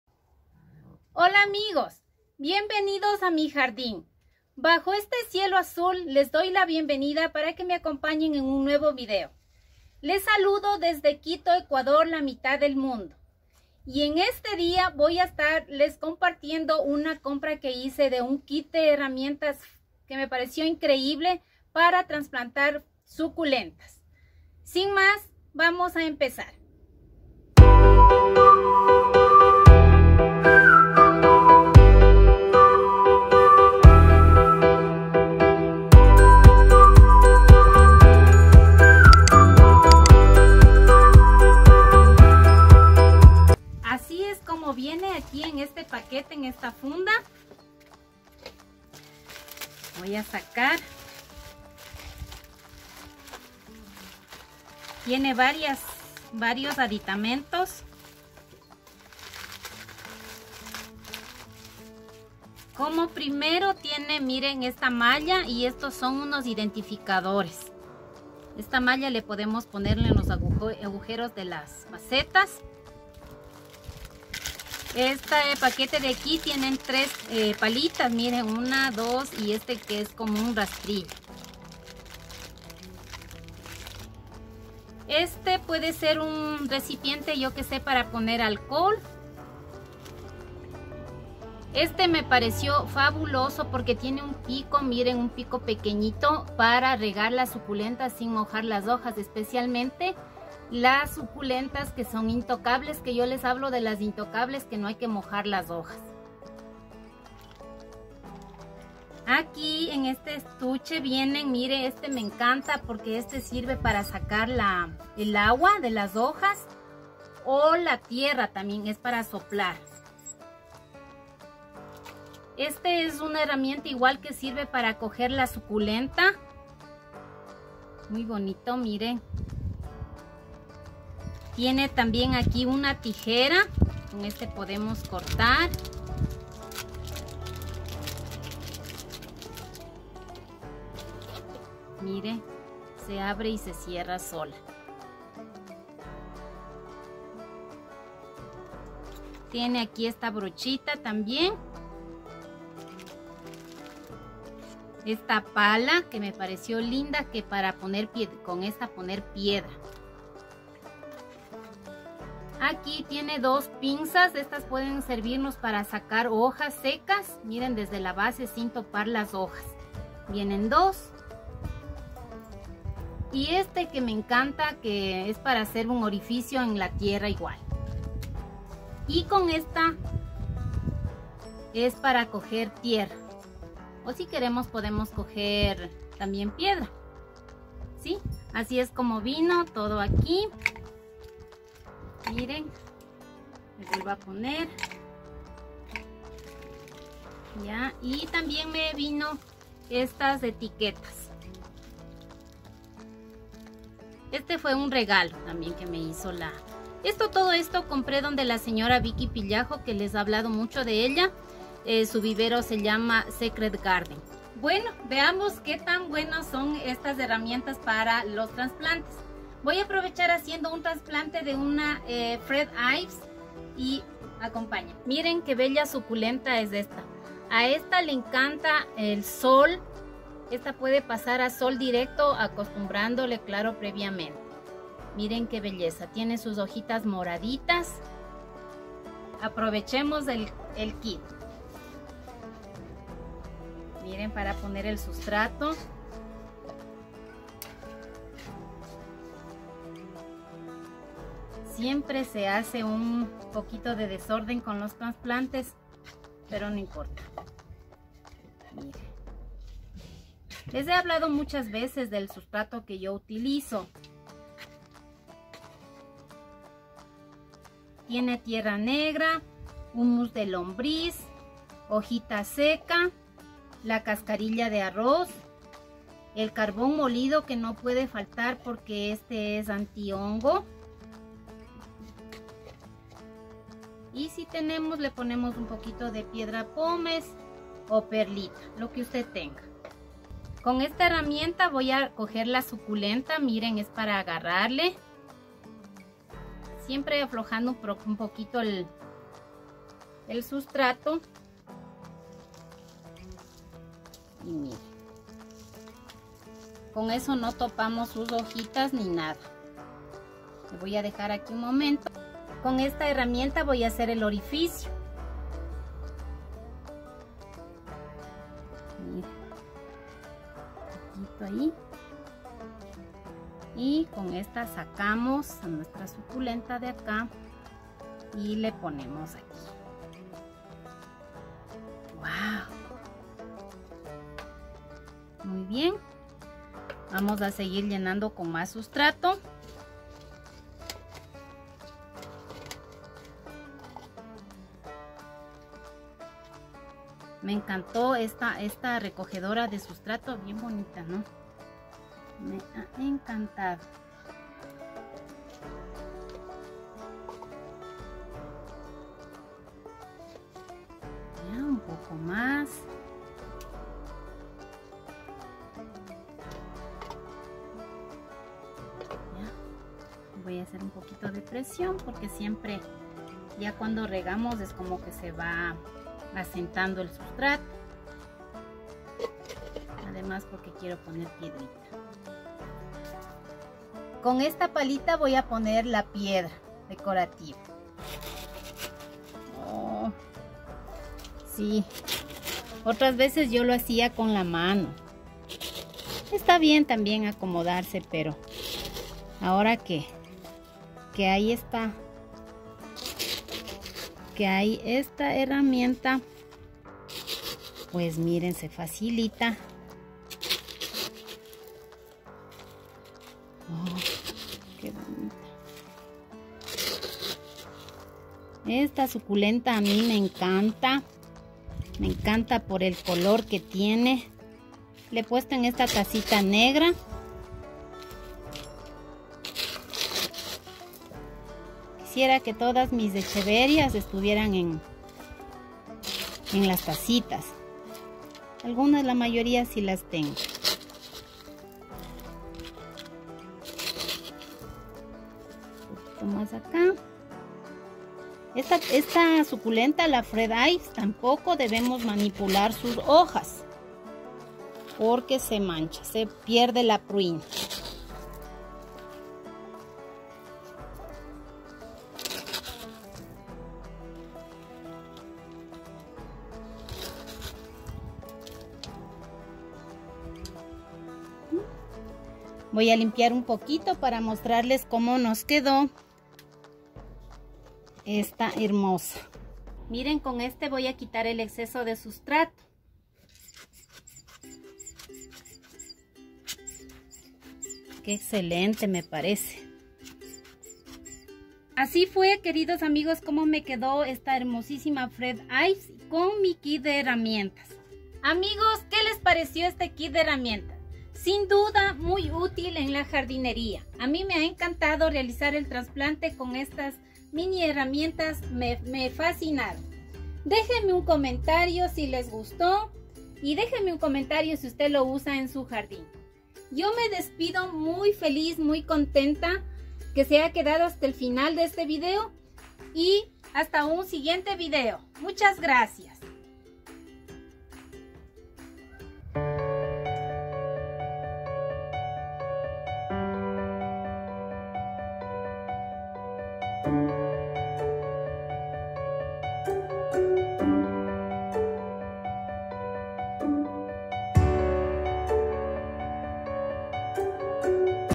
TRASPLANTANDO SUCULENTA CON MI NUEVO KIT DE JARDIN
Hola amigos, les doy la bienvenida a un nuevo video y en esta ocasión les presentaré un trasplante de mi graptoveria Fred Ives con un kit muy especial que estoy segura que desearán adquirir este kit pues ayuda a realizar trasplantes de todo tipo de planta sin dañarla y cuidarla en el proceso de su cambio a una nueva maceta, agradezco a mi amiga Vicky de "Secret Garden" que fue quien me ayudó con este kit, visitenla ya que tiene hermosas  suculentas que ella les podrá ayudar a adquirir. Disfruten del video y comenten que le les pareció este interesante kit.